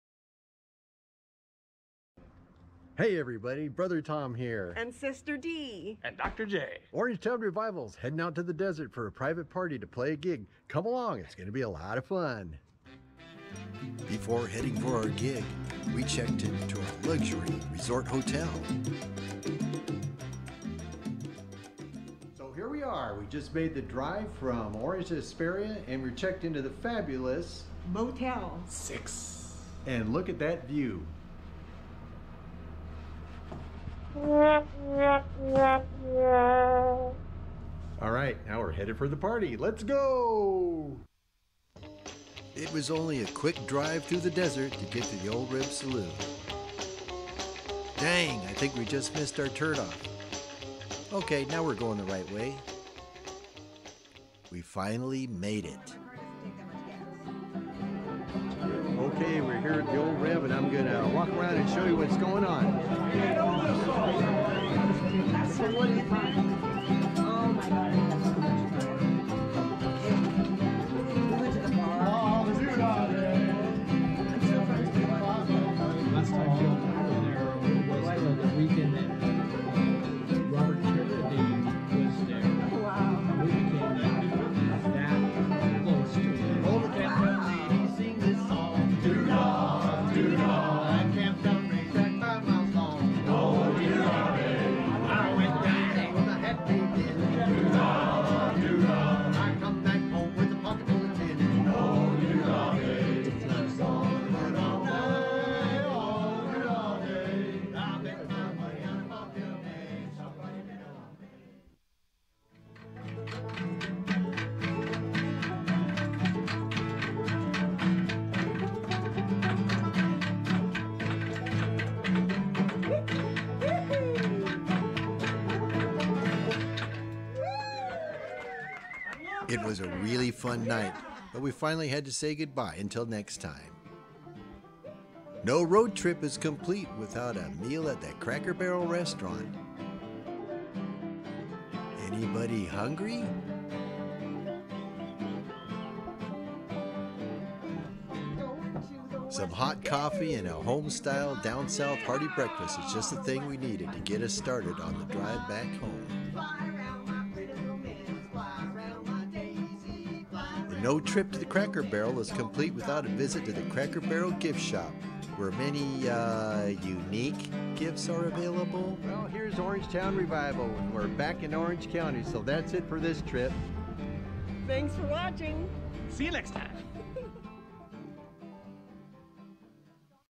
Saloon Nite 2023 Vlog - The Orange Town Revival
Brother Tom, Sister Dee, and Dr. J go on a road trip to Hesperia to perform at The Old Reb Saloon, Saturday, June 10, 2023.